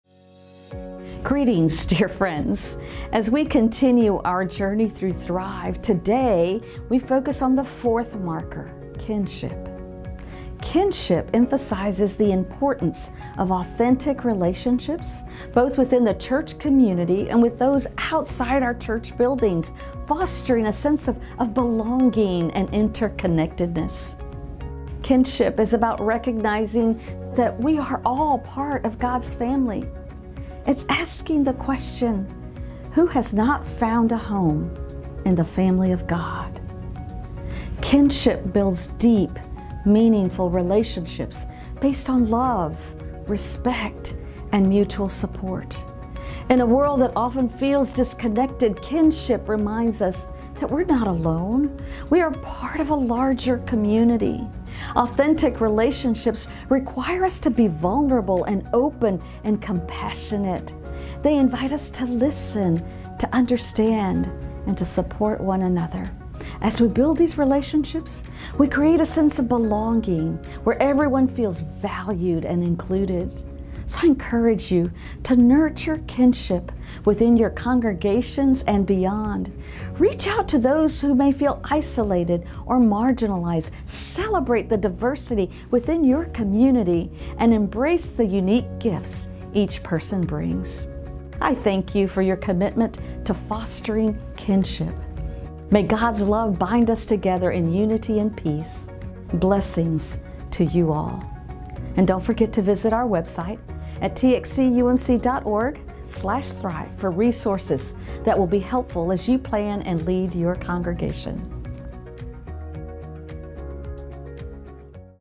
THRIVE: Kinship
As we continue our journey through THRIVE, today we focus on the fourth marker: kinship. Kinship emphasizes the importance of authentic relationships, both within the church community and with those outside our church buildings, fostering a sense of belonging and interconnectedness.
Kinship is about recognizing that we are all part of God's family. It is asking the question “who has not found a home in the family of God?” Kinship builds deep, meaningful relationships based on love, respect, and mutual support. In a world that often feels disconnected, kinship reminds us that we are not alone—we are part of a larger community.
Authentic relationships require us to be vulnerable, open, and compassionate. They invite us to listen, understand, and support one another. As we build these relationships, we create a sense of belonging where everyone feels valued and included.
I encourage you to nurture kinship within your congregations and beyond. Reach out to those who may feel isolated or marginalized. Celebrate the diversity within your community and embrace the unique gifts each person brings.
Thank you for your commitment to fostering kinship. May God's love bind us together in unity and peace. Blessings to you all.